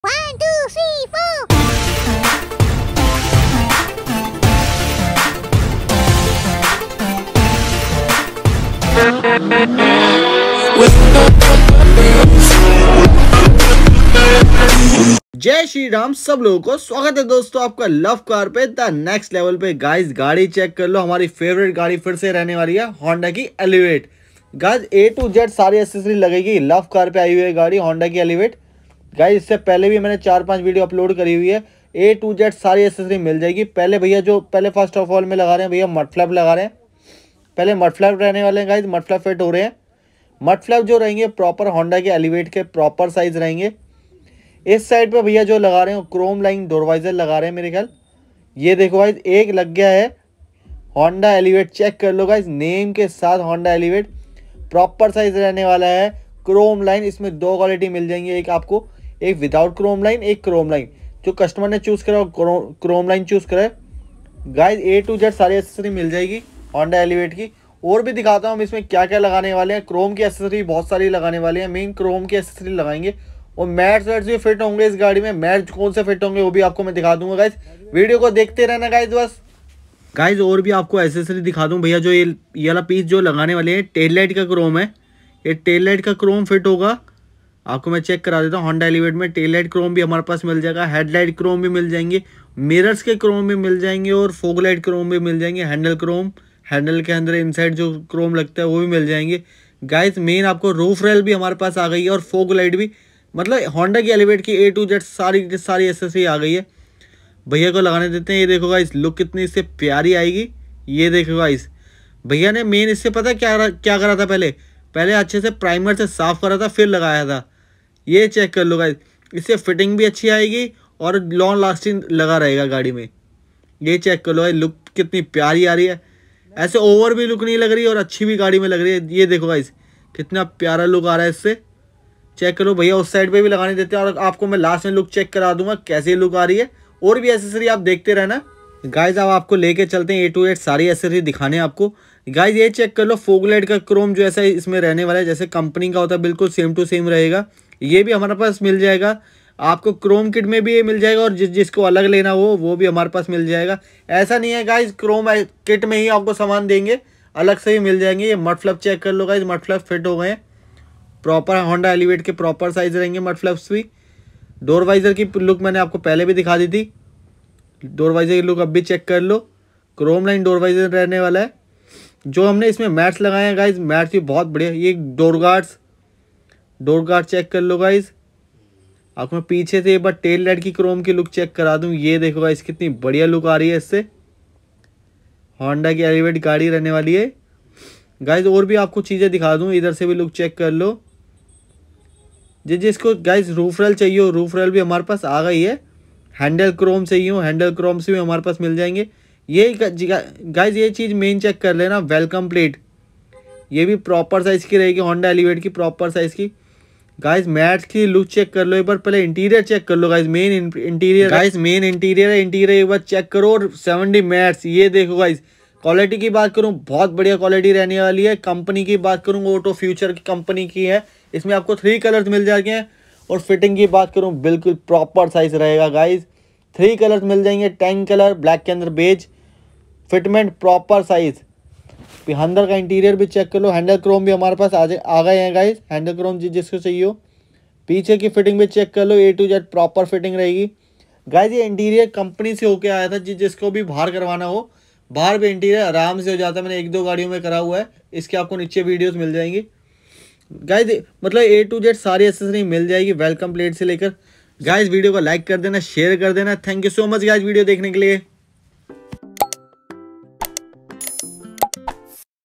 Honda Elevate Seat Covers🔥Mats || Elevate Crome Accessories || Honda elevate SUV
Hello friends watch this video to see and know About all new Honda Elevate with look and details including interior & exterior !!!

This video is all 2023 Honda elevate accessories available which includes Doorwizar , seat covers , mats , DRL , stering cover , dicki crome , welcome , Doorwizar , android sterio , speaker, bumper protecter, crome ,foglglamp ,footrest etc

all Accessories Available - Luv car

LUV CAR accessories
Near by RBL BANK
Address-56,57 Shastri market , Gurudwara Road